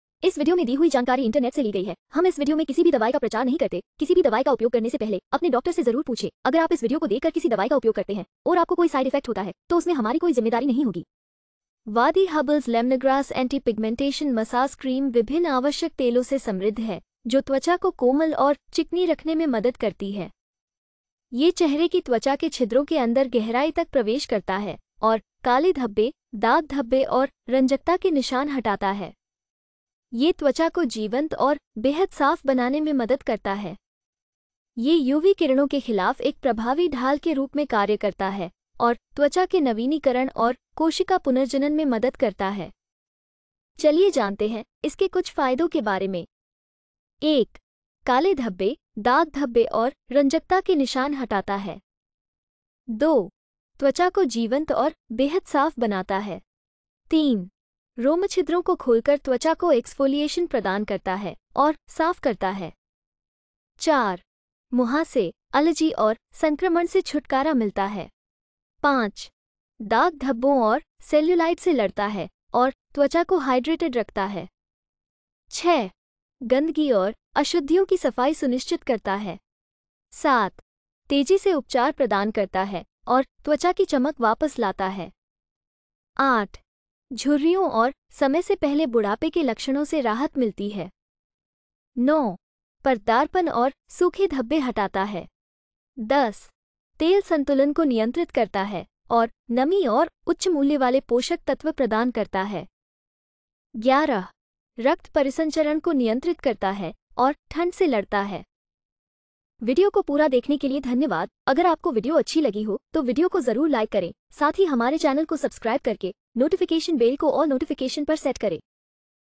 Vaadi Herbals Lemongrass Anti-Pigmentation Massage Cream Uses in Hindi | Side Effects | Dose
Vaadi Herbals Lemongrass Anti-Pigmentation Massage Cream Uses in Hindi | Benefits | Side Effects | Dose